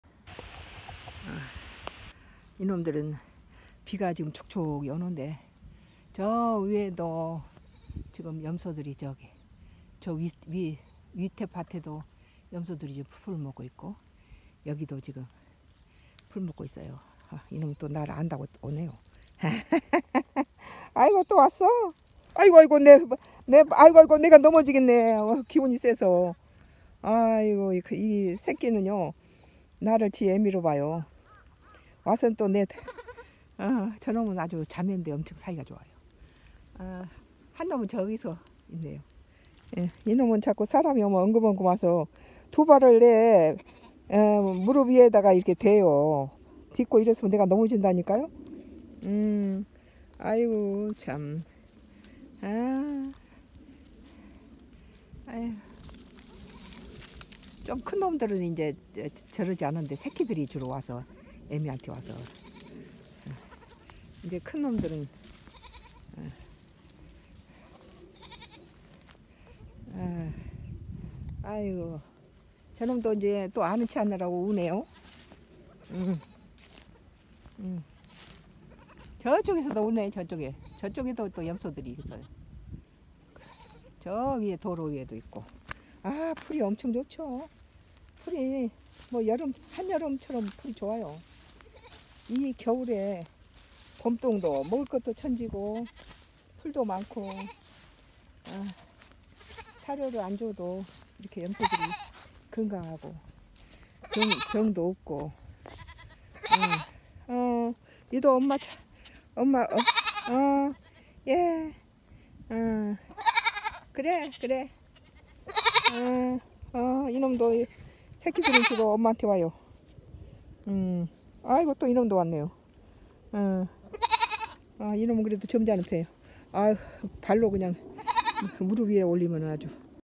가랑비 내리는데~~~풀밭에 풀뜯는 염소들~~~!@sungodcross
가랑비 내리는데~~~풀밭에 풀뜯는 염소들~~~!

가랑비 내리는데~~~! 풀밭에서 풀뜯는 우리염소들~~주인이 나타나자`~
반갑다고 배에 두발을 얹고~~~함께 놀자고 붙잡는 염소 새끼들~~~!
비맞은 풀들은~~~땅밑에서 초록색으로 기운차게 솟고 있다~~~! @sungodcross